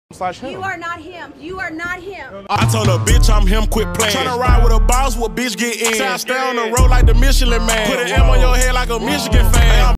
Best Bullpen in Baseball (but not him) 🤣 Go Dodgers 🔥
Thank you Dodgers Pitchers Evan Phillips, Julio Urias, Alex Vesia, Yency Almonte, 🚨(NOT YOU CRAIG KIMBREL), 🚨 Tony Gonsolin, Clayton Kershaw, Brusdar Graterol, and Tyler Anderson for being the best pitching staff in the MLB 👏